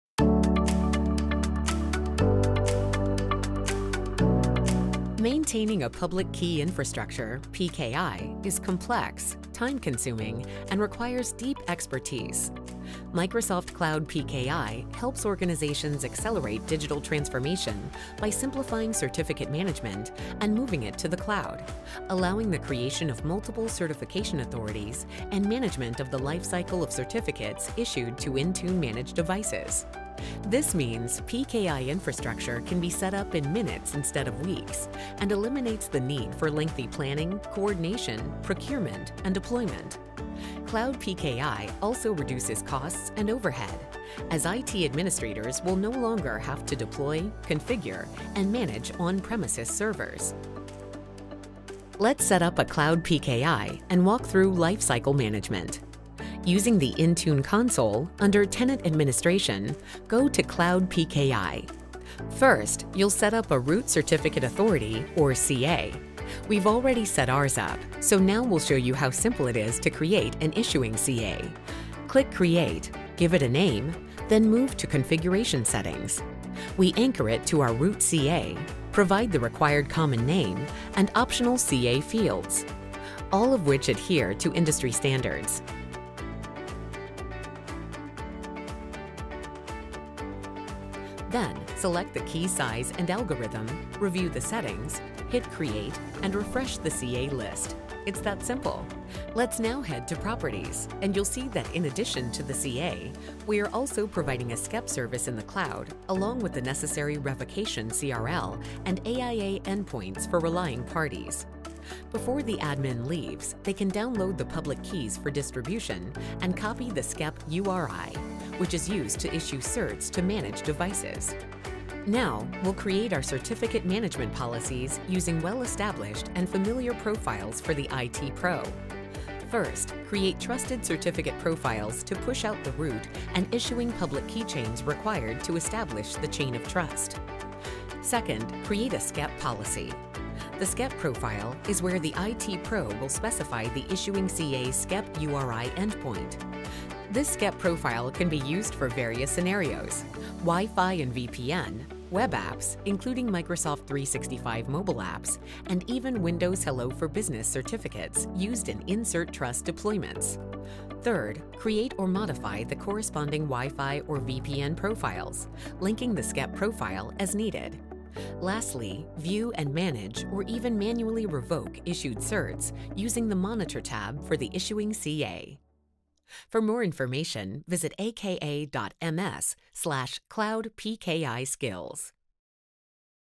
Microsoft Cloud PKI
Microsoft Cloud public key infrastructure (PKI) simplifies and automates the certificate lifecycle management for Intune-managed devices. It's a cloud-based service that provides a dedicated PKI infrastructure for your organization, and doesn't require on-premises servers, connectors, or hardware. Learn how to set up a Cloud PKI, and get a walkthrough of the lifecycle management.